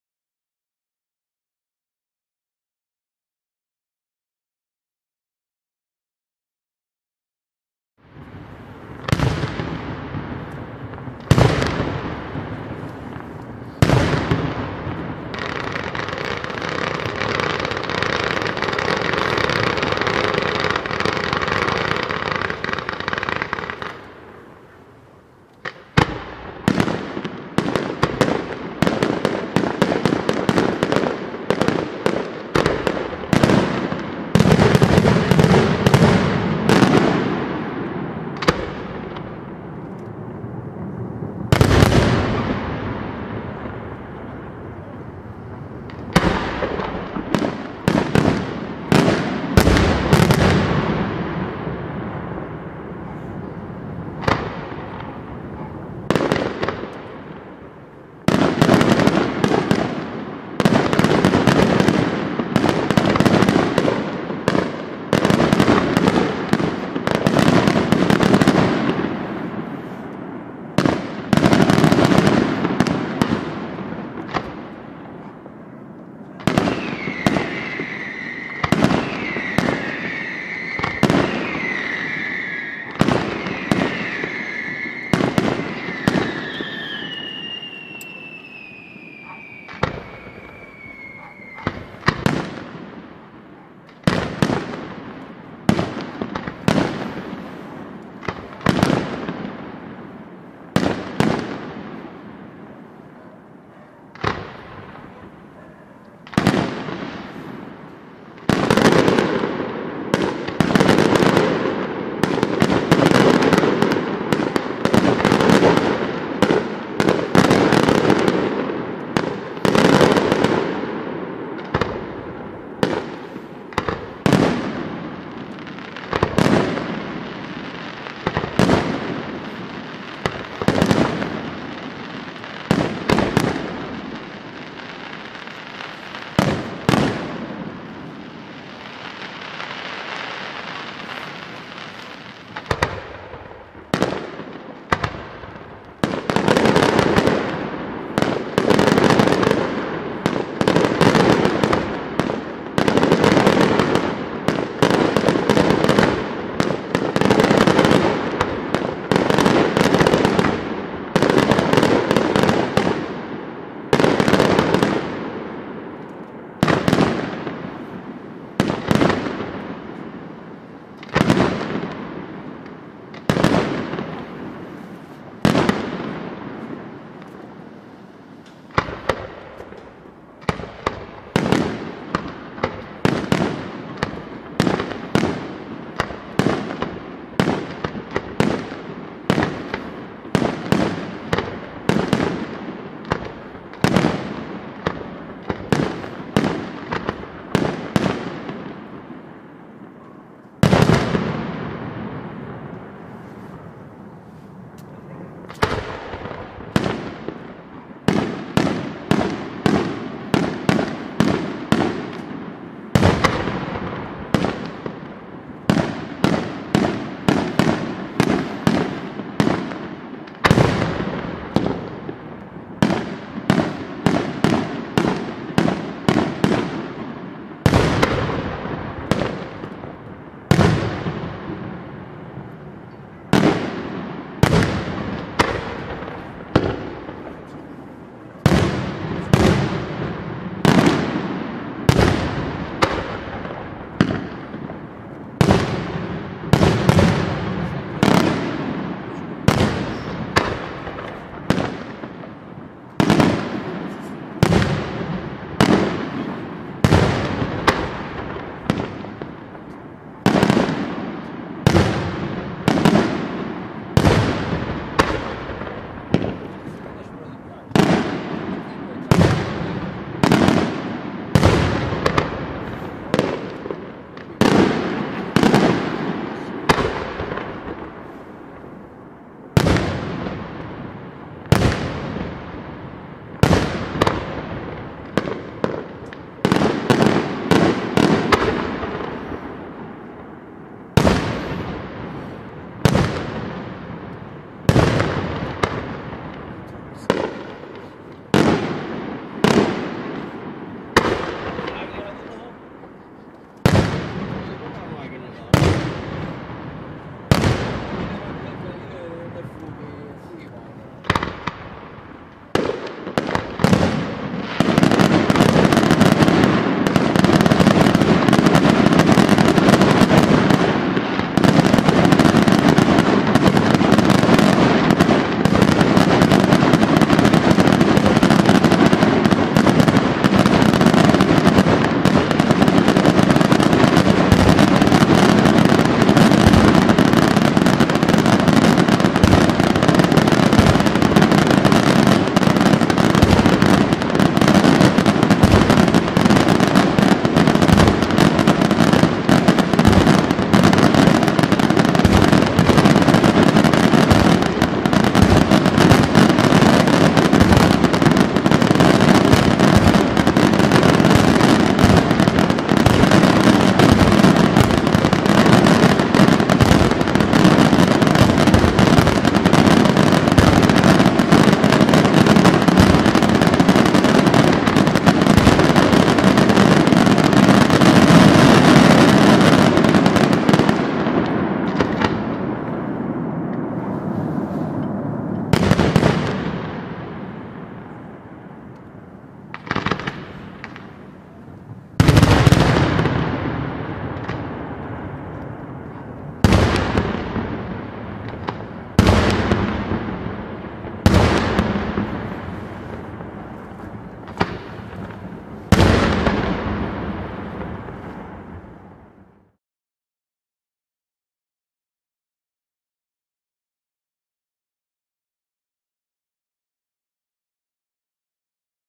Spettacolo Pirotecnico Notturno in onore di S. AGNELLO, Roccarainola 2022 - PIROTECNICA CAVOUR
Roccarainola (La) Loc. Gargani - 19 Dicembre 2022
Festeggiamenti in onore di Sant'Agnello Abate
Spettacolo Pirotecnico Notturno - Ass. Sant'Agnello Abate
PIROTECNICA CAVOUR di Vincenzo Boccia da Piazzolla di Nola (Na)